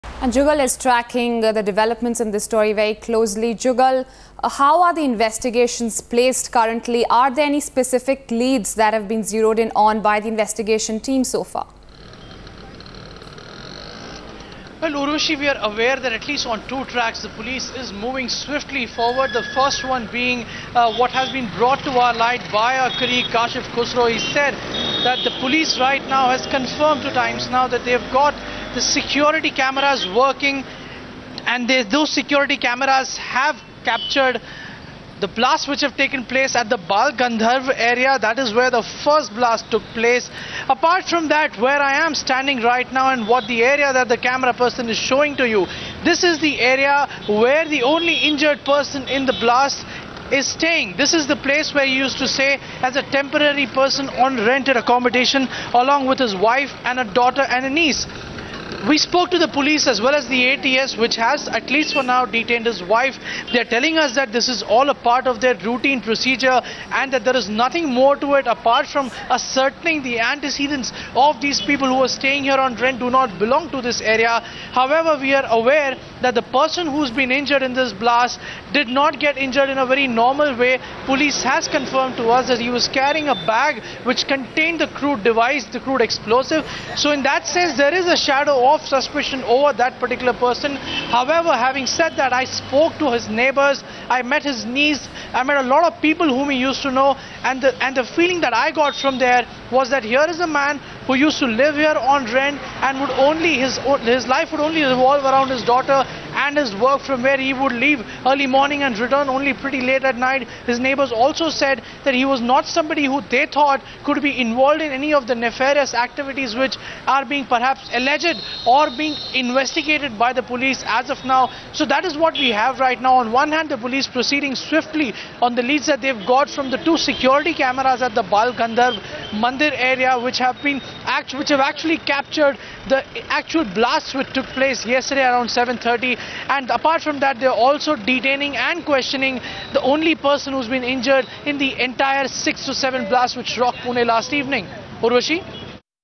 Central investigators begin probe
According to official sources on Thursday (August 2) the Pune police have said that they do not have any leads on the 4 low intensityu blasts that took place in the heart of the city last night. Police sources added that they have sought assistance from central agencies in the ongoing probe into the incidents. Download the Times Now India's Election HQ app and get all the election info at one go. Social Media Links